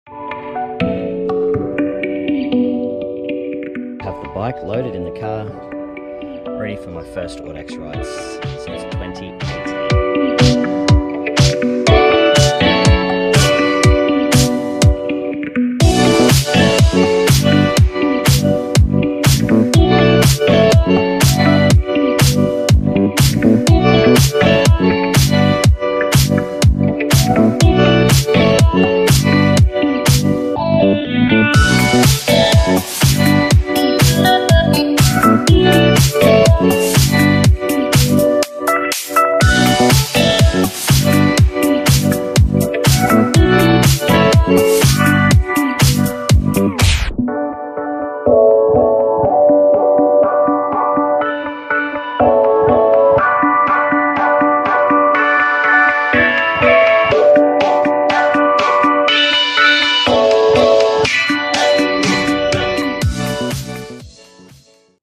Flat Fifty Brevet Permanent
After four years away from the sport, I’ve decided it’s time to return to Audax cycling. January 2013 will mark my ten year anniversary of my first ride with the Audax Queensland club but I’ve only been an active member for a few years with breaks between.

To start my return, I’ve completed the Flat Fifty Brevet Permanent today (19 October 2022). This was also my first ride when I returned after a long break in 2018.

I had a lovely few hours out on the bike.